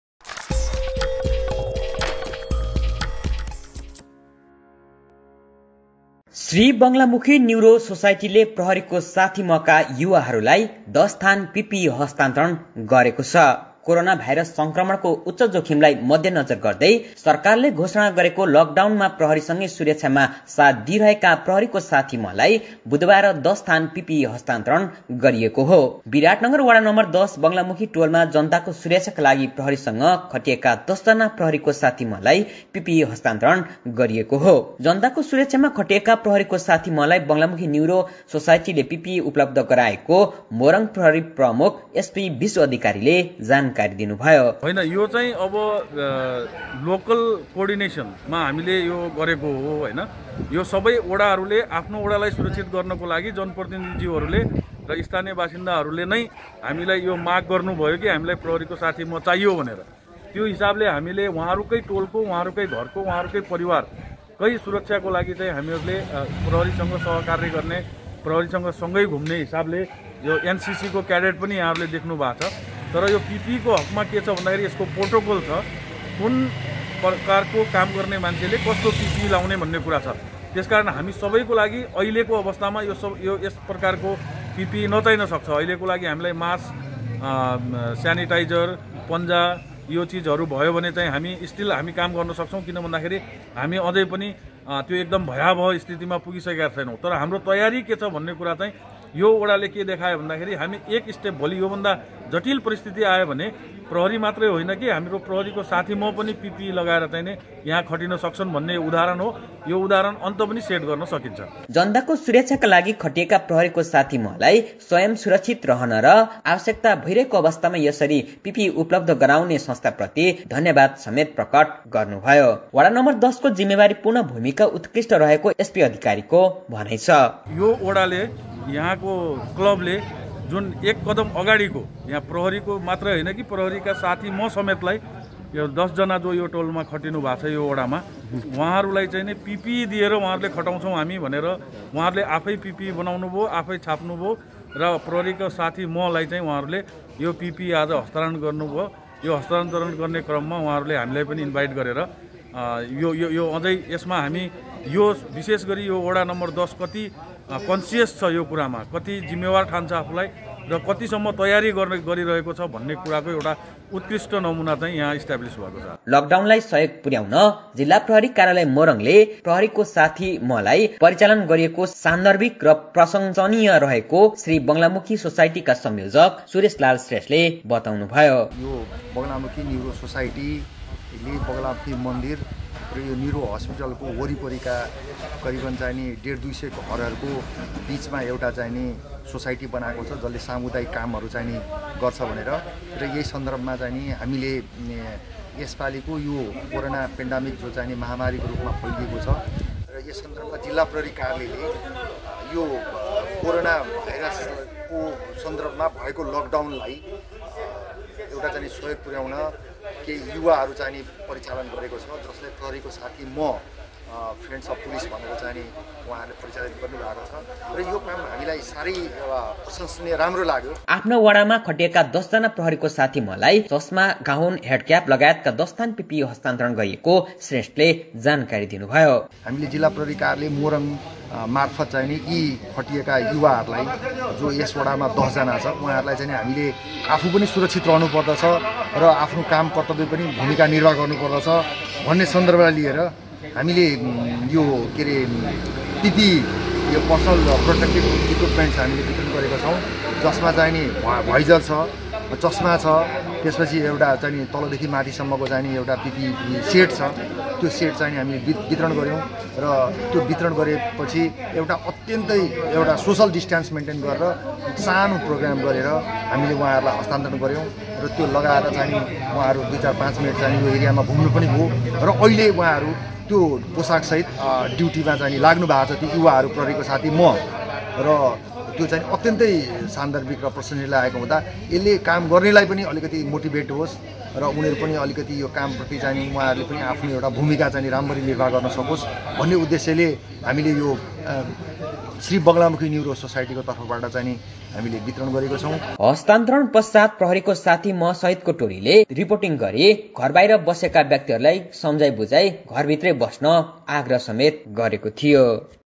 'प्रहरीको साथी म' लाई १० थान पिपिई सेट
विराटनगर वडा नम्बर १० मा खटिने 'प्रहरीको साथी म' लाई १० थान पिपिई सेट